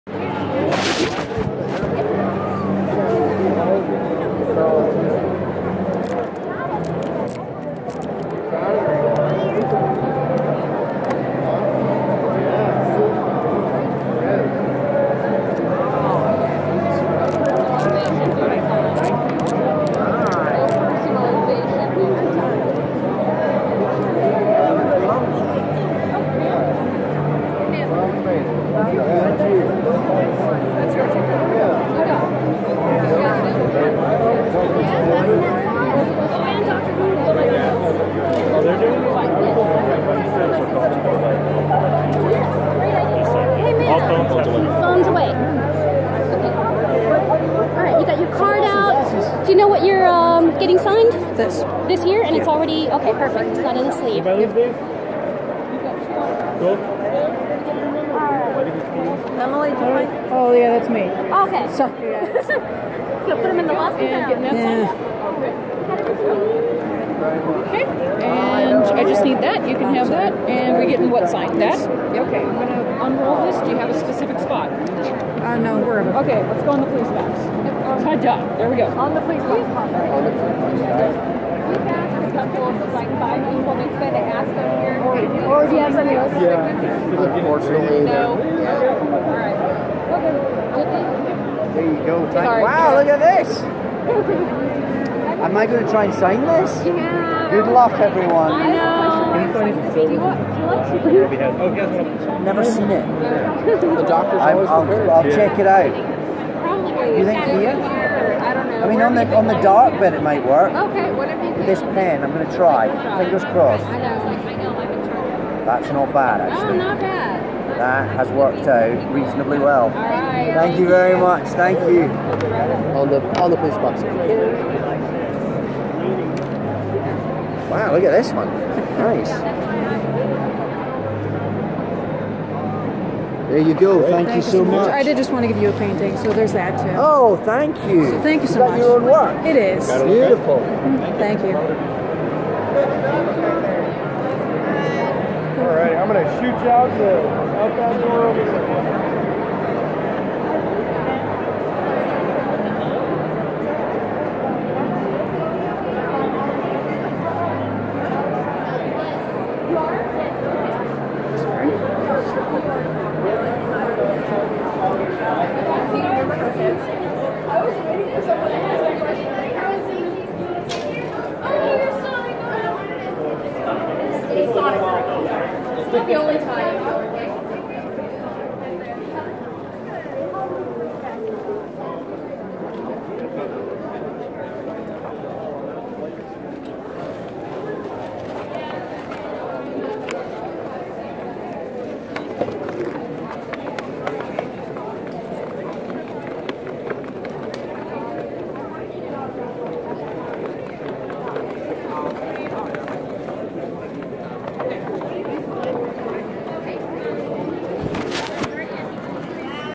Meeting David Tennant
David Tennant at his autograph table at Wizard World St. Louis 4/2/2016